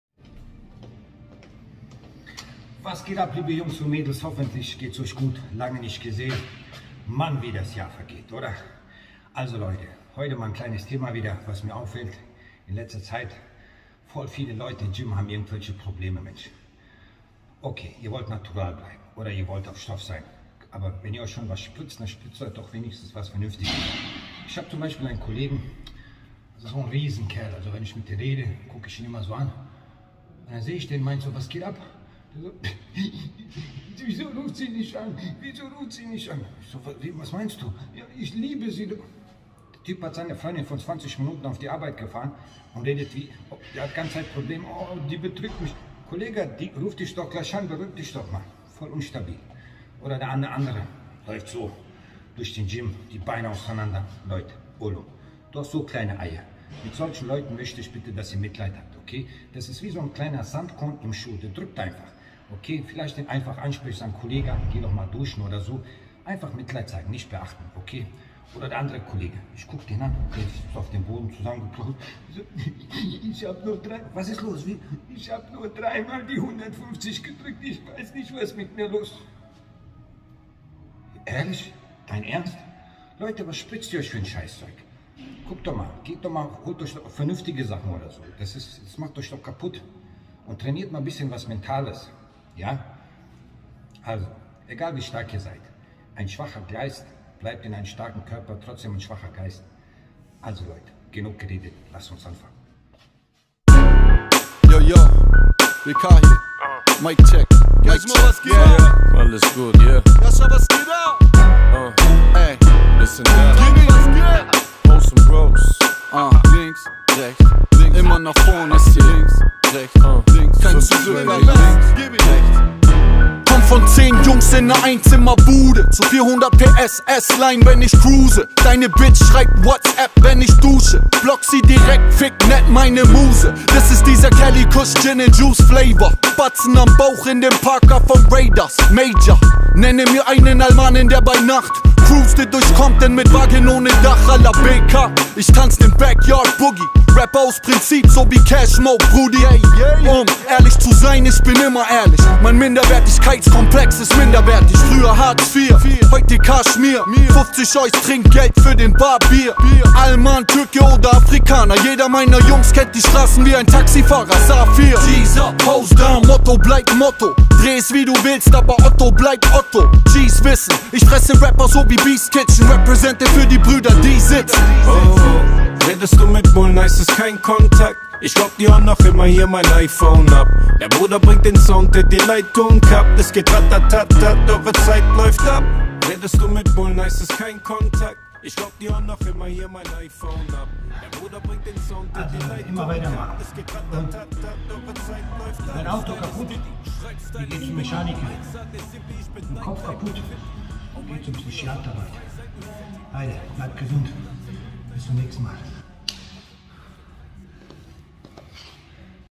strong minds don't need heavy weights
Wenn auto kaputt ihr geht doch zum mechaniker.Was macht ihr wenn ihr mental nicht stark genug sein ,dann geht doch zum Psychiater. Immer mehr Leute begegne ich in meinem Leben die  richtig kaputten Kopf haben .
Immer nur an jammern, Immer die Schuld bei anderen suchen,nicht mal in der Lage sich zu entschuldigen wenn sie was falsch gemacht haben .Geht zum Arzt Leute oder zum Schamanen
Ihr braucht Hilfe. Ihr seid Männer und keiner Kinder mehr ,wenn ihr jammern wollt ,geht ins Schwimmbad  oder im see ,taucht 2 Meter tief und  jammert euch da die Seele raus .
An alle anderen nicht nur Körper trainieren sondern auch den Geist.
Bleibt stark
Bleibt gesund
Und denkt positiv